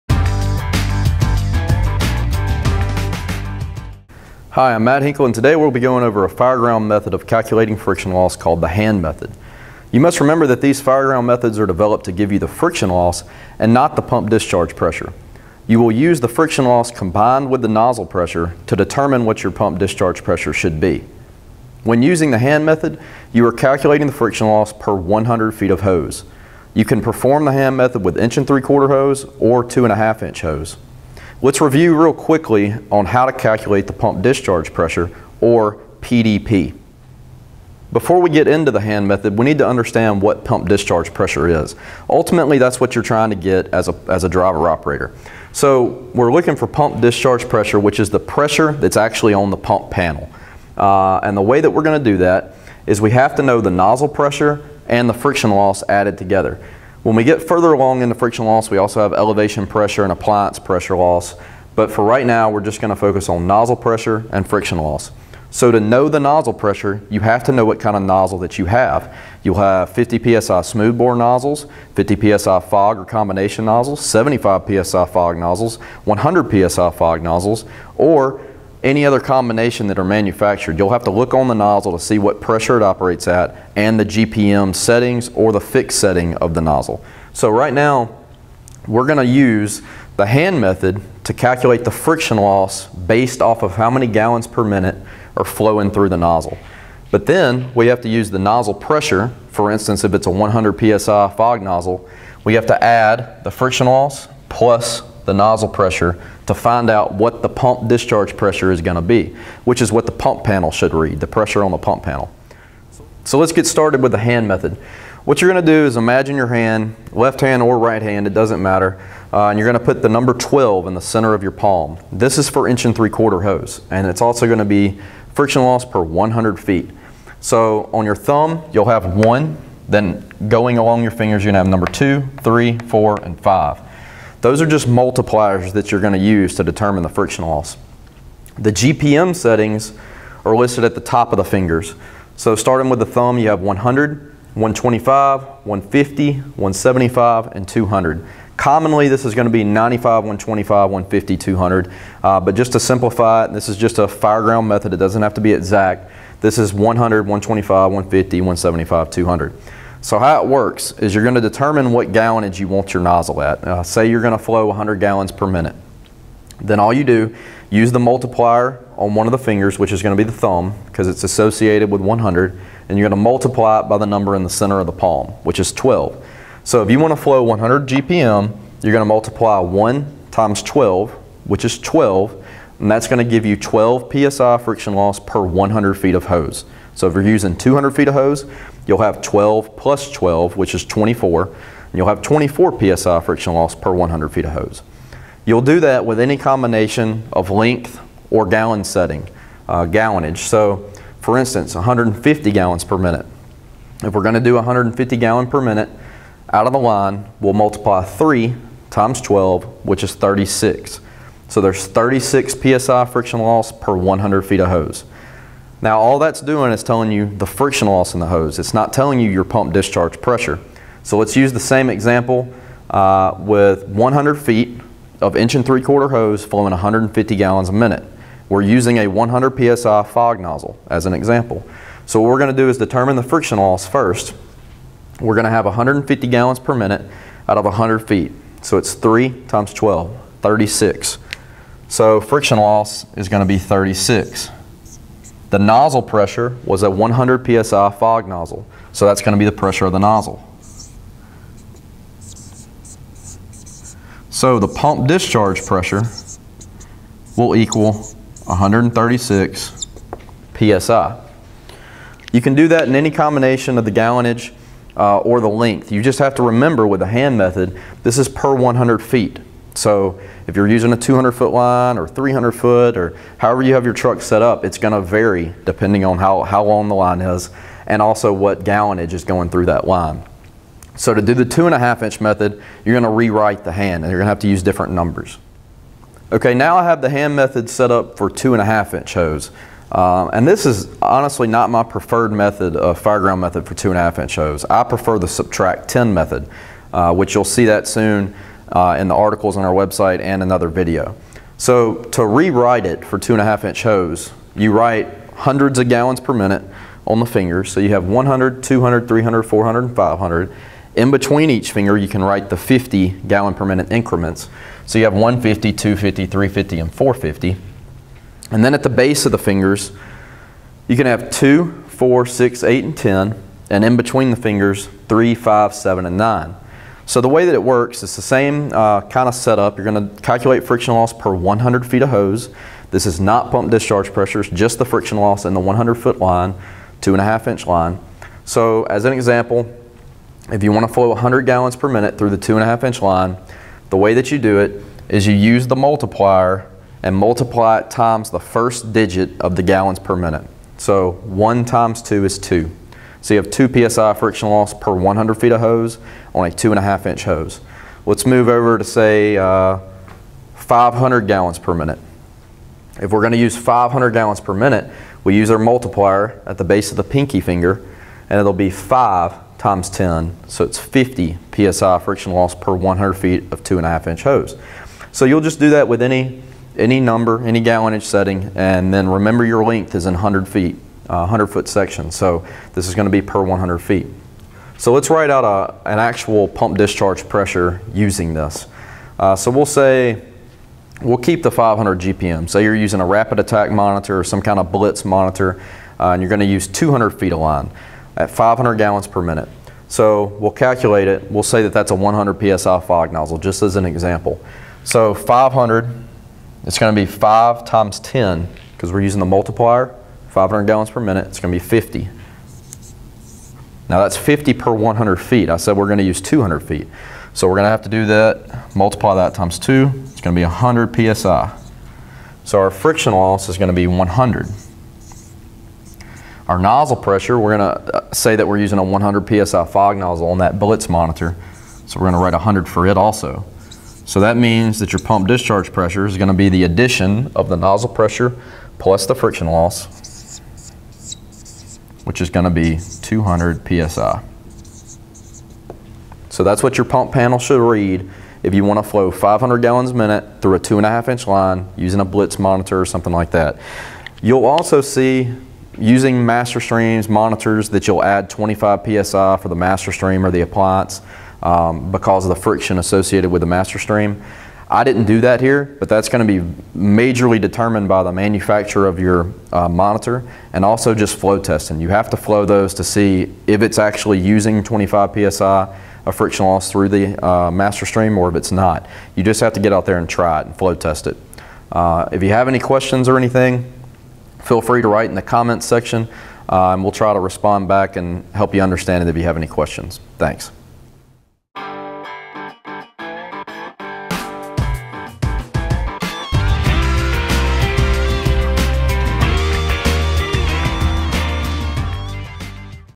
Hand Method for Calculating Friction Loss for Firefighters
This video is a review of how to calculate friction loss for firefighters using the hand method. This is a fireground method of calculating friction loss for 1.75" and 2.5" hose. To see more training resources make sure you visit our